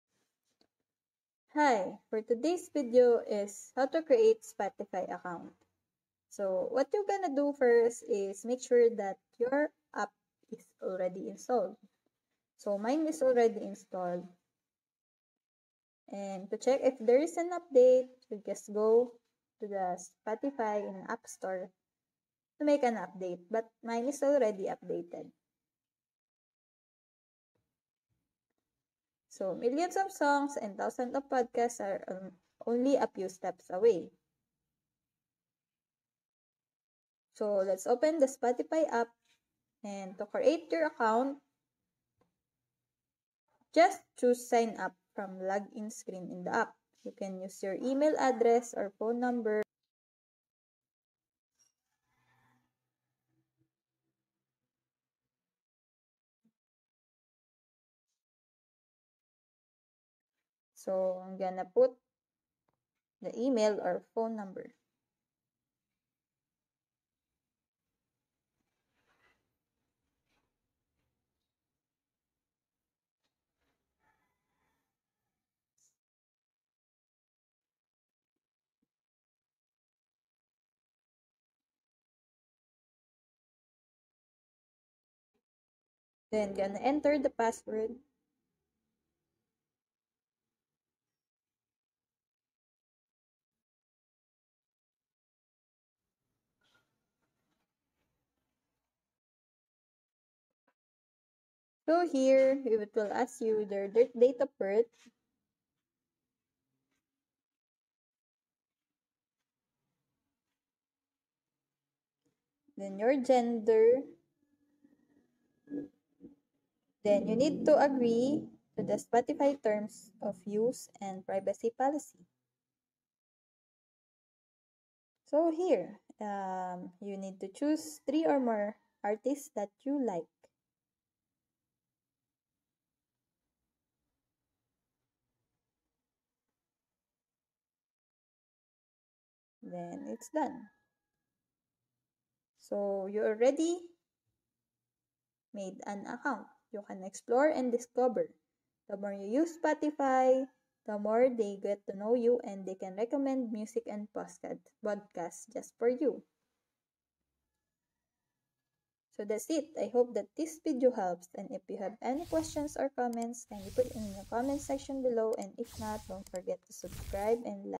How To Create A Spotify Account On Android | Social Tech Insider
In this tutorial video from Spotify, we explain to you the best way to Create A Spotify Account On Android.

💡Want to know something and this video is not yet on our channel?

📸 Our videos are created to help people with their problems online.